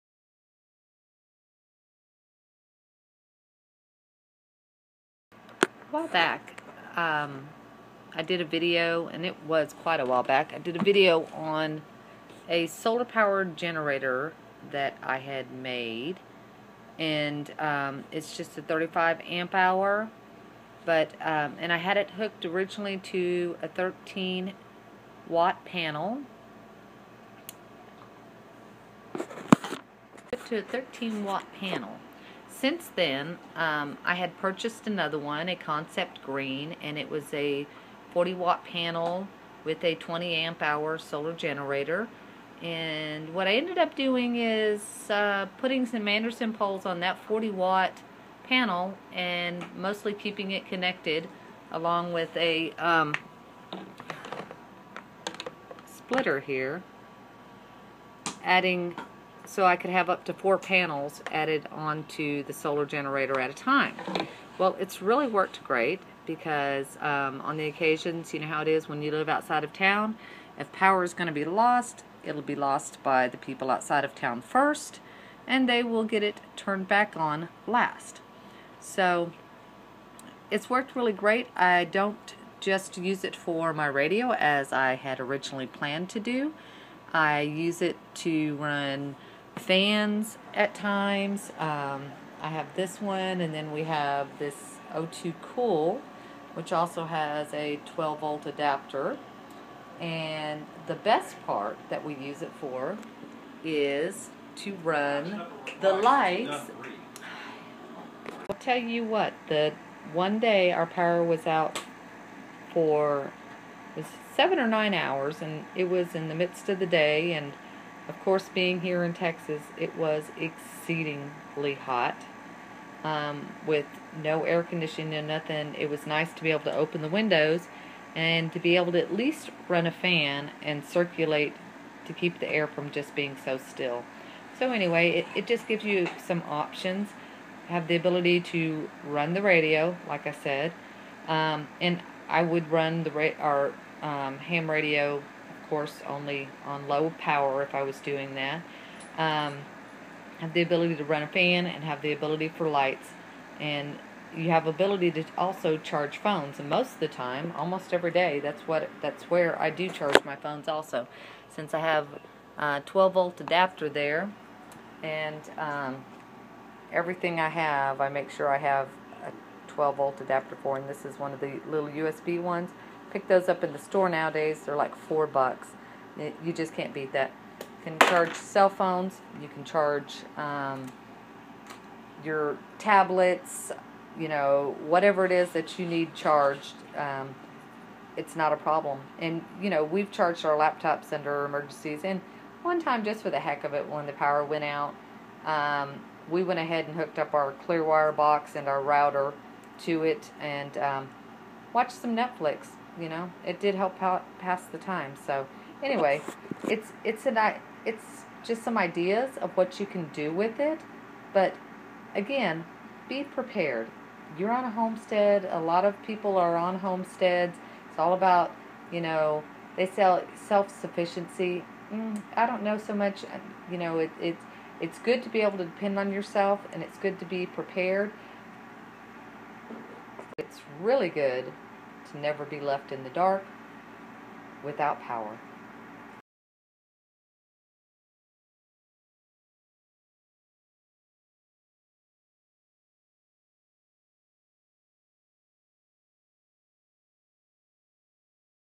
Prepper Power - Homemade Poratable Solar Generator Update
A quick update on how the homemade portable solar generator is working out after one year.  Here's a ling to the original video